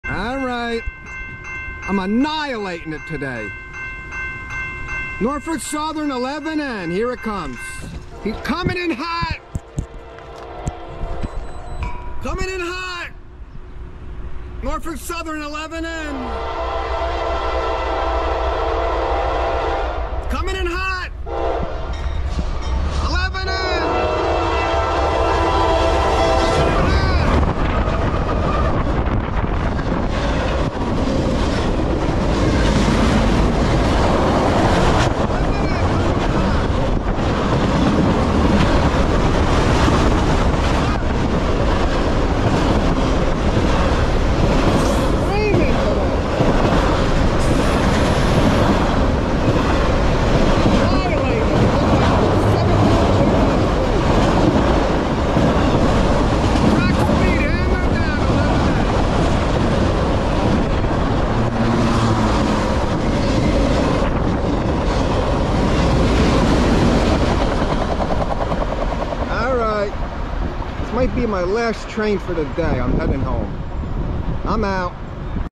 [4K] NS Train 11N 50 MPH Coming in hot with some Auto-Max Cars- Sound On!
South Plainfield, NJ-  Norfolk Southern Autorack Train 11N Screaming at track speed with #4525 (GE AC44C6M)  leading #8083 (GE ES44AC) with some new badass Auto-Max rail cars.

Railfans I have something for everyone! :-)
My playlists are updated almost daily.
ALL are greatly appreciated.  Thank you and God Bless :-)